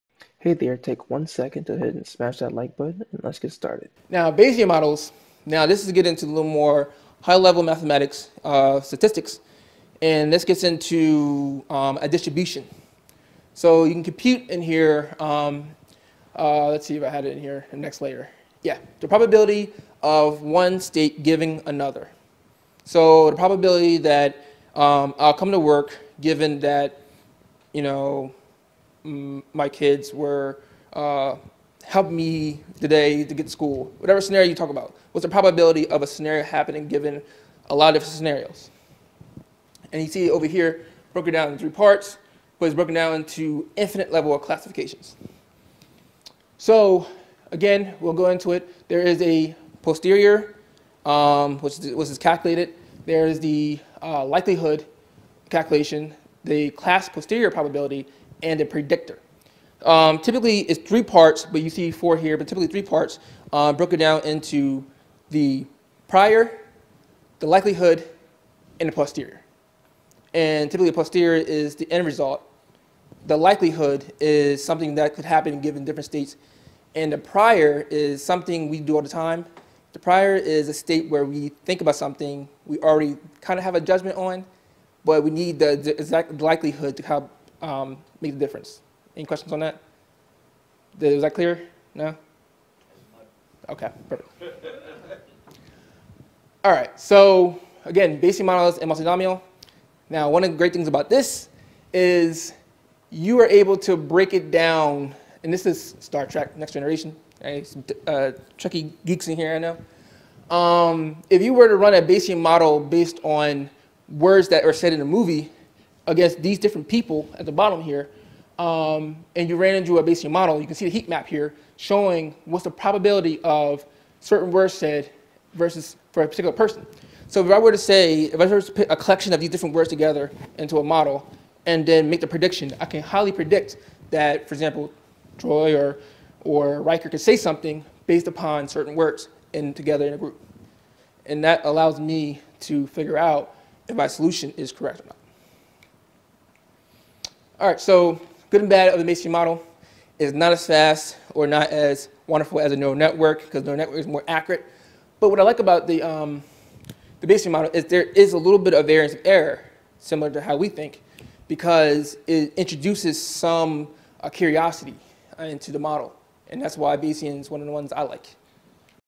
Bayesian optimization AI models
Discusses Bayesian Neural Network.  We talk about calculating the Posterior Distribution by understanding the  priori and the likelihood calculation

We begin the Thinkmoat 2020 AI an AGI Meetup
We go over the entire AI landscape and then we
discuss AGI and the approach that we could take.